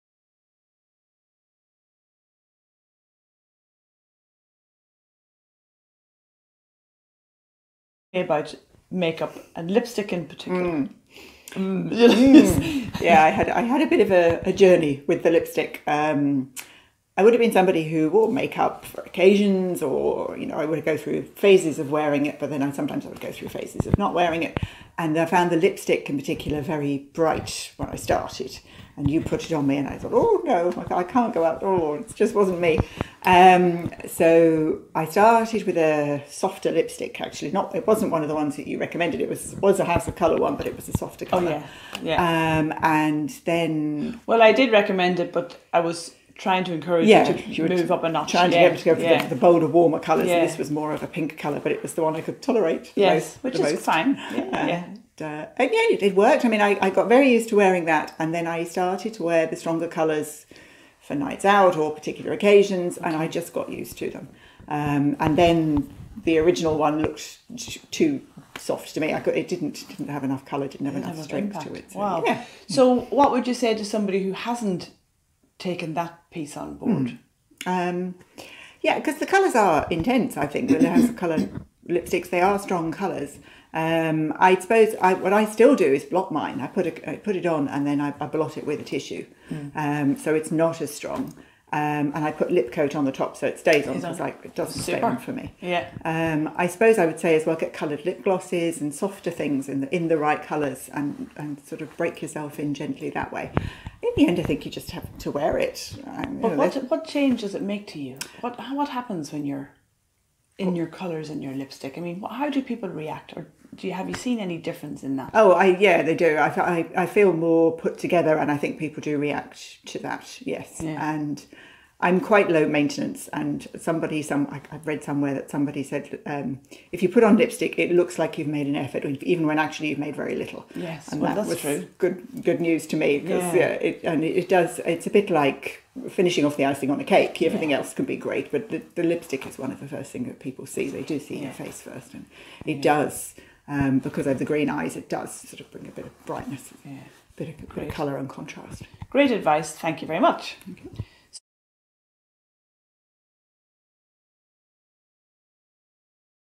Sofa Session Part II Lipstick and Make up
Part of every colouranalysis class is discovering the best foundation, blusher and lipsticks. Every client embraces this in her own way. Research states that when wearing lipstick, you are perceived to be more competent and credible.
In this sofasession, Ceinwen reveals how she made the move to wearing lipstick every day.